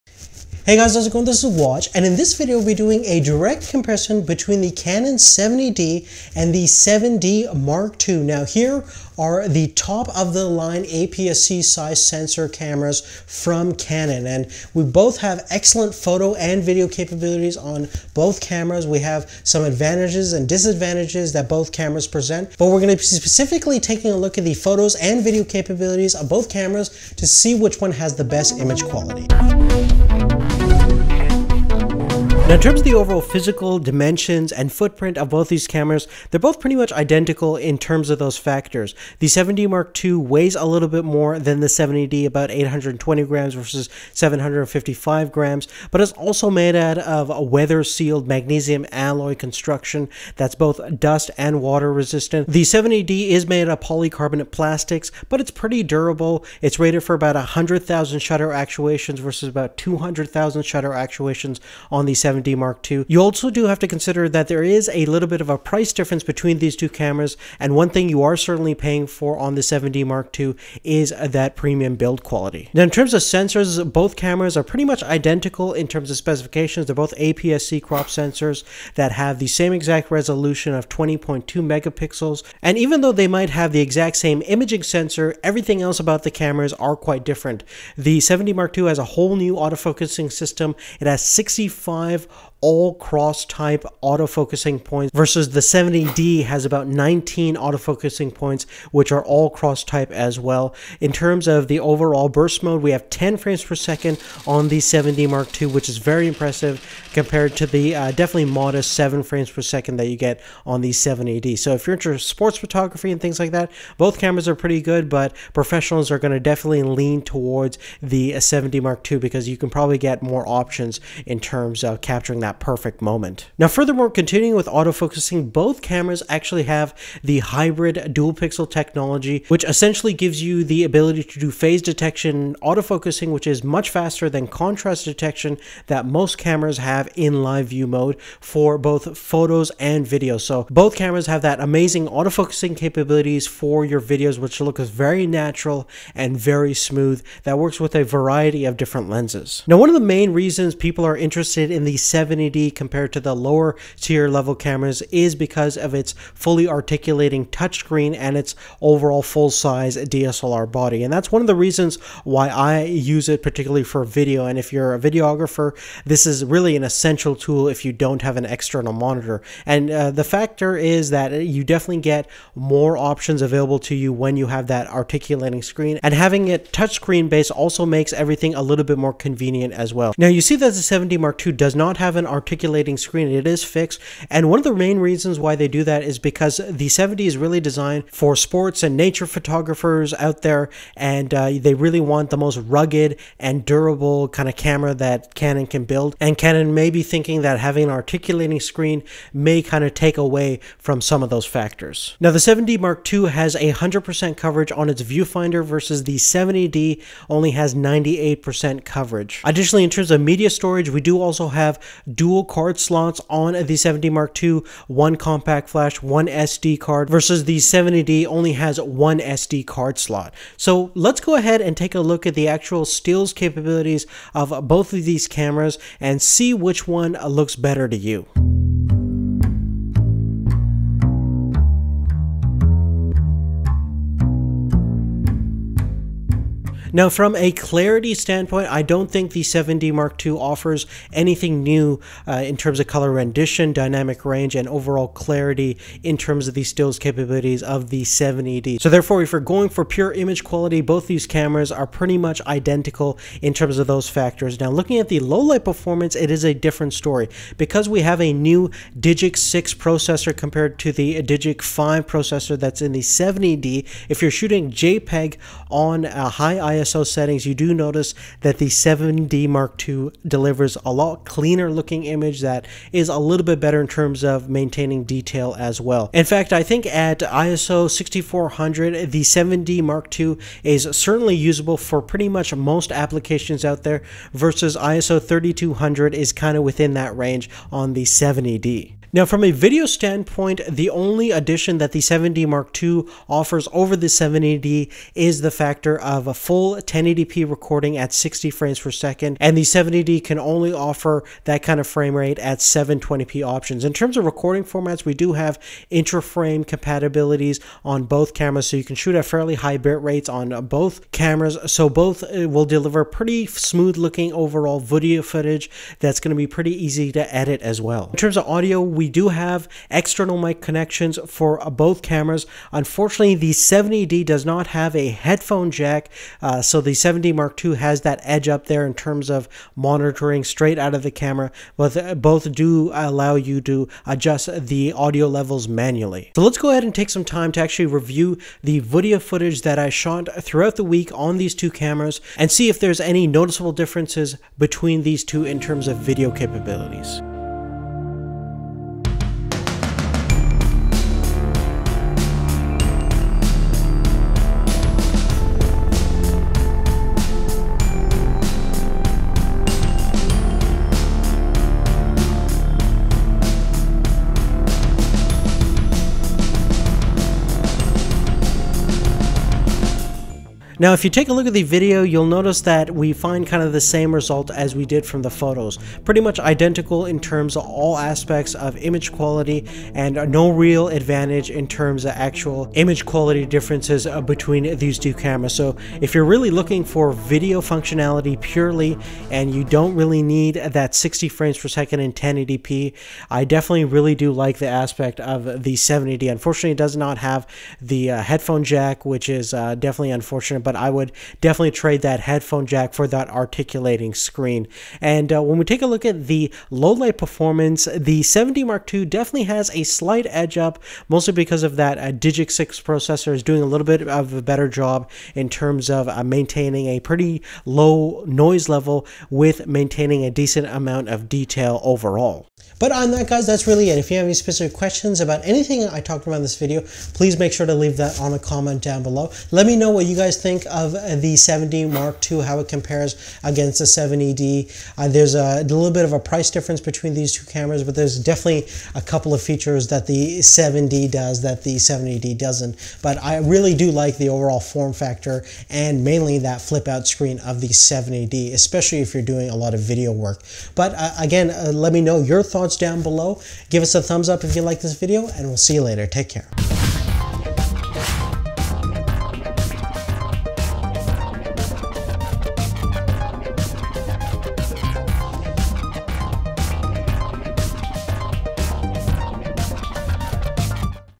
Canon 7D Mark ii vs Canon 70D Full Comparison
Here is our Comparison of the Canon 7D Mark ii vs the Canon 70D.

Help make our video possible by using are our links when you get

Amazon US
Amazon Canada
Amazon UK